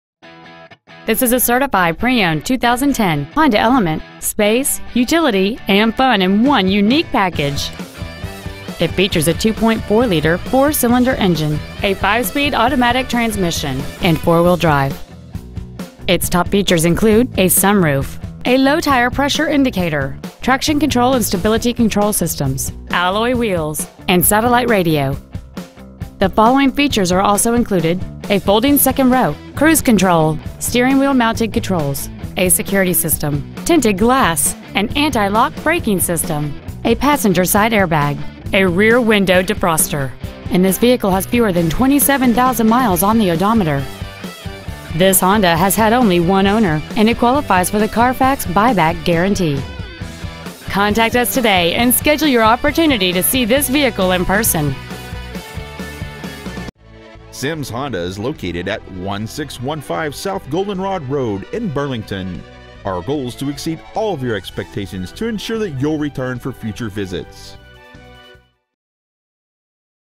2010 Honda Element Certified Burlington WA 98226
We are proud to present this 2010 Honda Element .

 Year : 2010
 Make : Honda
 Model : Element
 Engine : 2.4L I4 DOHC 16V
 Trans . : 5 Spd Automatic
 Exterior : Red
 Miles : 26,388
 Interior : Gray
 Sims Honda
 1615 S. Goldenrod Road
 Burlington , WA 98233
 Used 2010 Honda Element Burlington WA